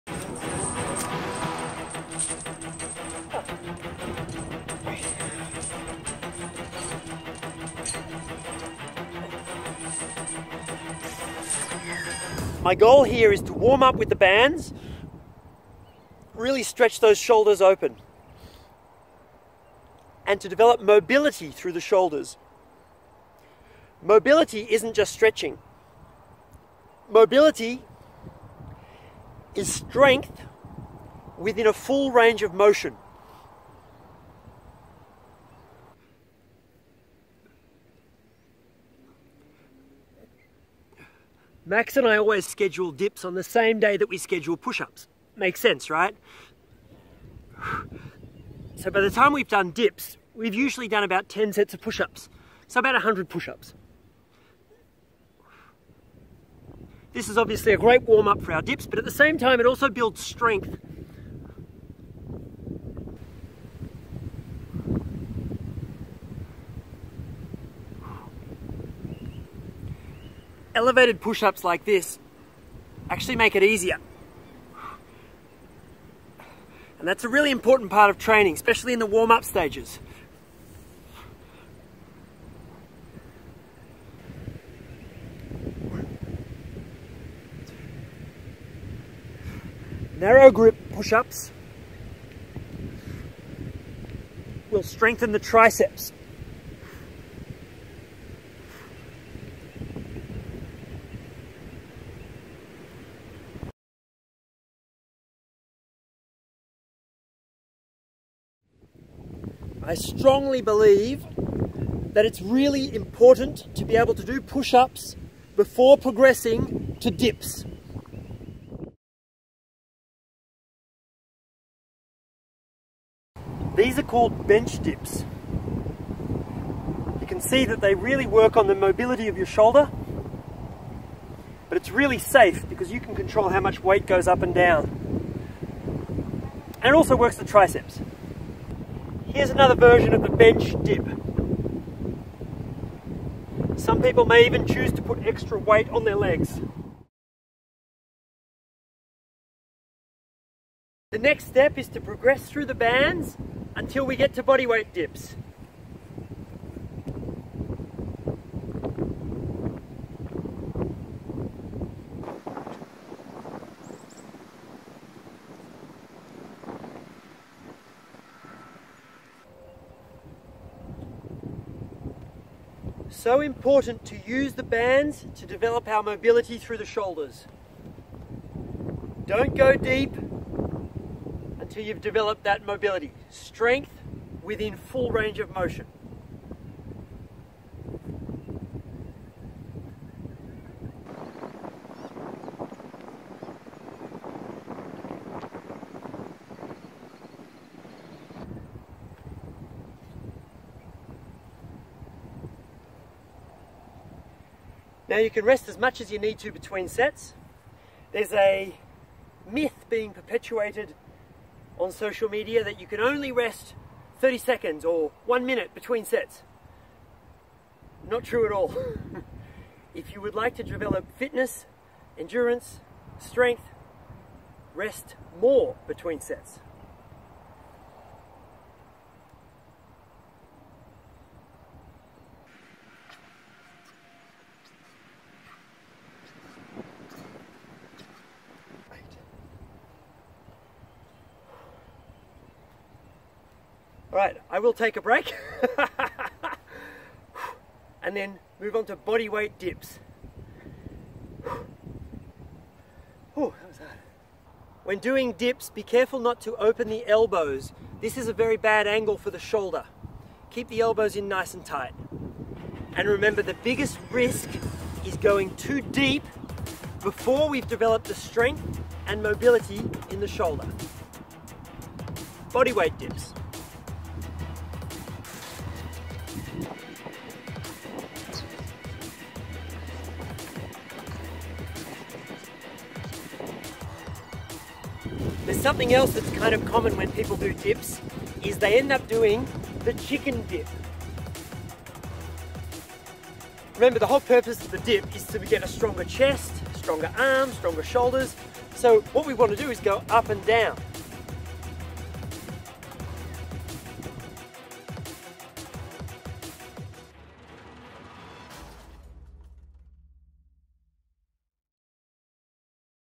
Don't Dip Deep - Mobility first
Dips are a great exercise for pecs, triceps and shoulders. To avoid injury, gain strength through push ups and use bands to develop some shoulder flexibility before attempting body weight dips or loaded dips.